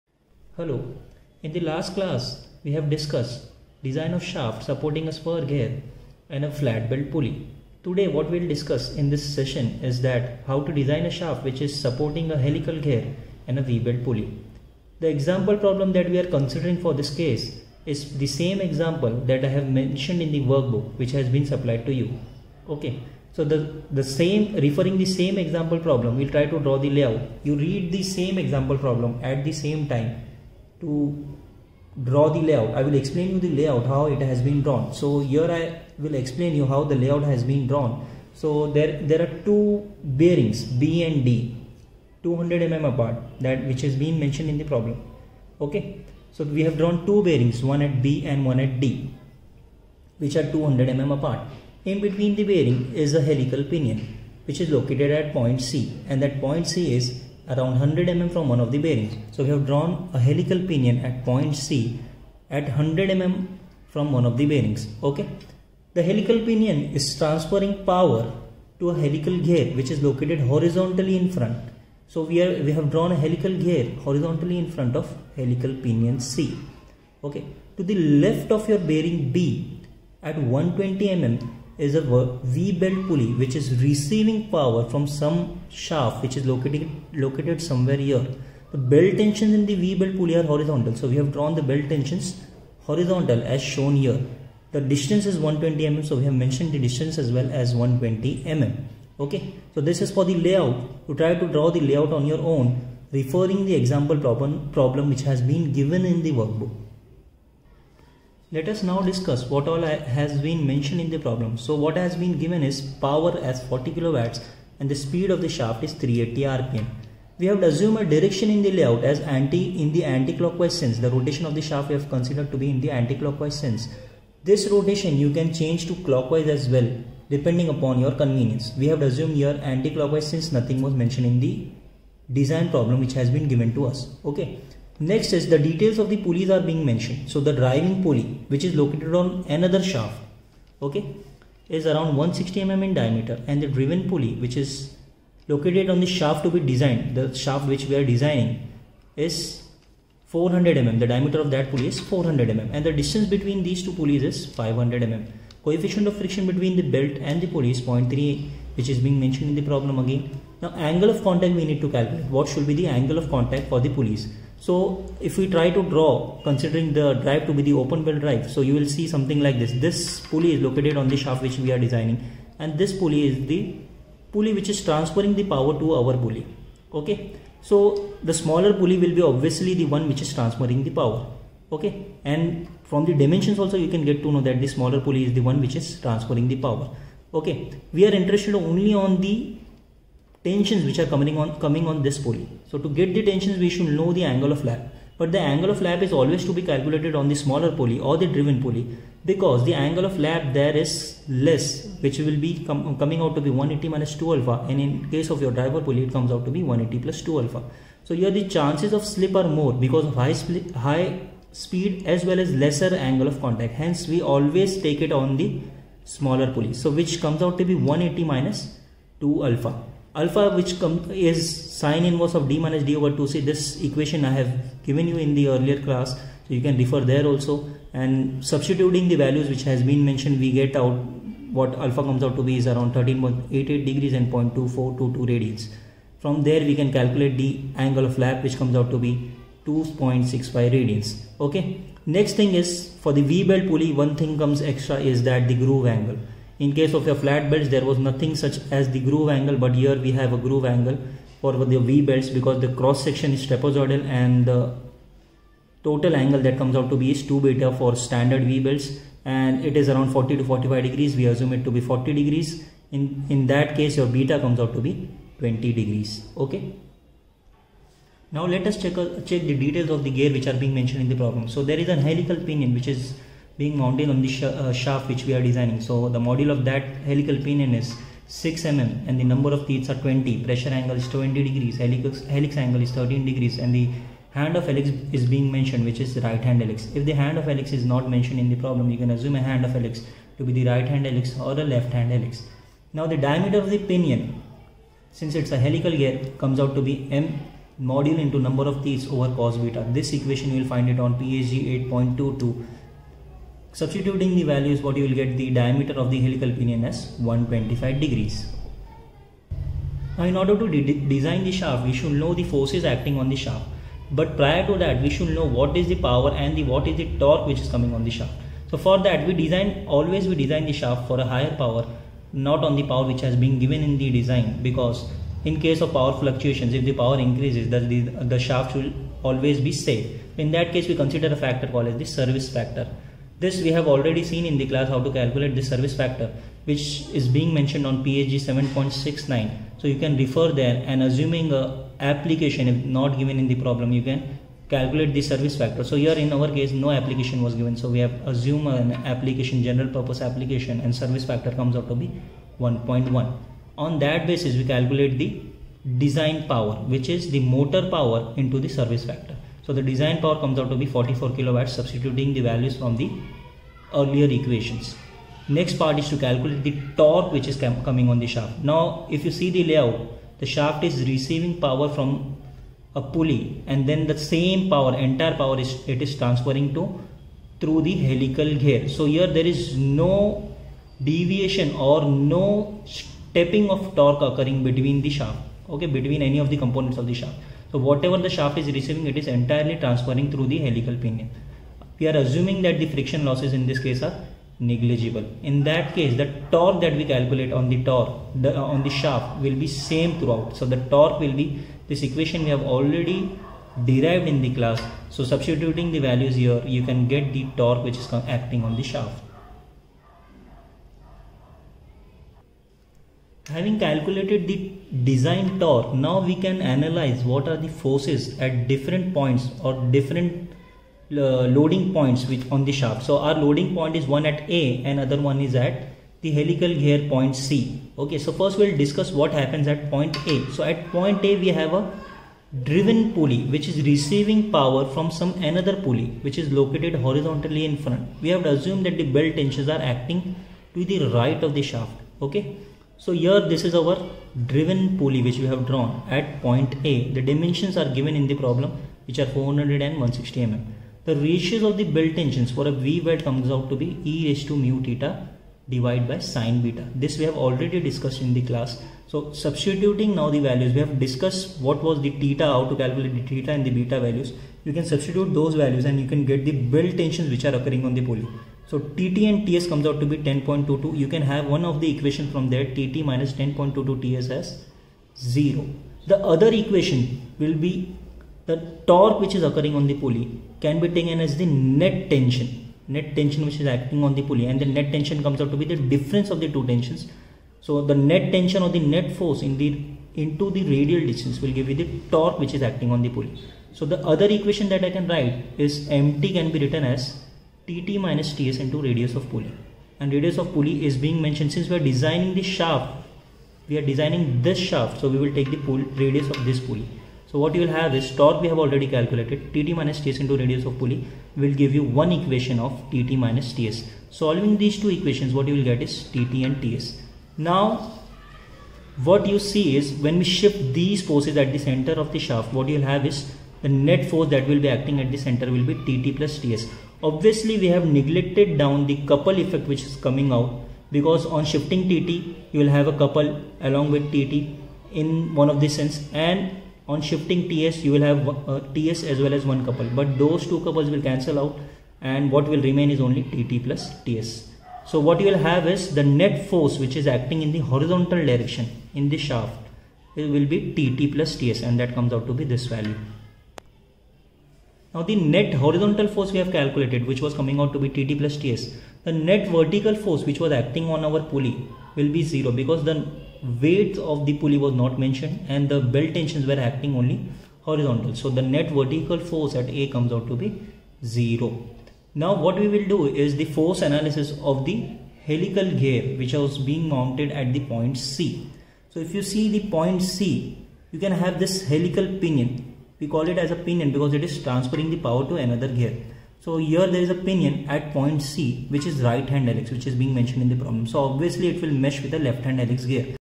DESIGN OF SHAFT PART-1
Design of shaft supporting a helical gear and a v belt pulley